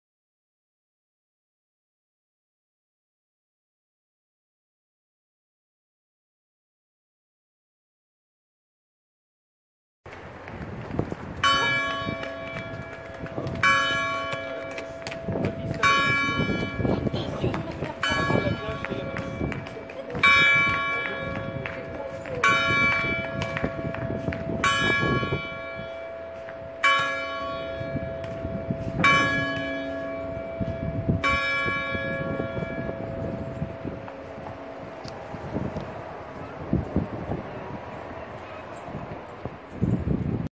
VAL-ANDRÉ [F] - röm. kath. Kapelle Notre Dame - Uhrschlag
Stundenschlag der Kirche Notre Dame in Val André, Frankreich.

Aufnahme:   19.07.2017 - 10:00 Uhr